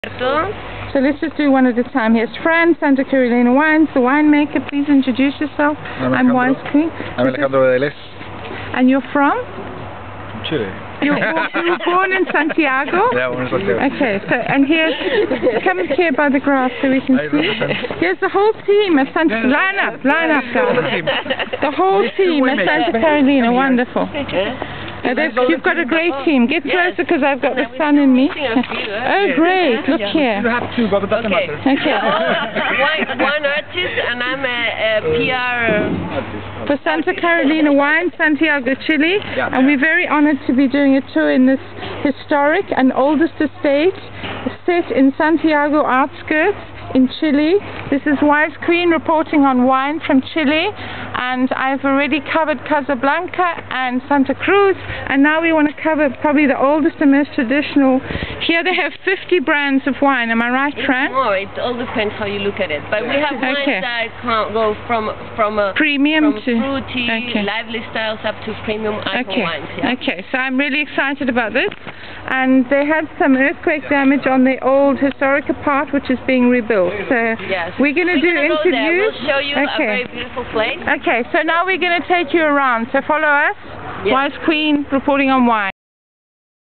wisequeen with winemakers Santa Carolina wines Chile, Andrés Caballero
The team of winemakers  at Santa Carolina wines Chile, with PR Francesca Martin  and the  head winemaker Andrés Caballero who I interviewed on his icon wine Herencia Carmenére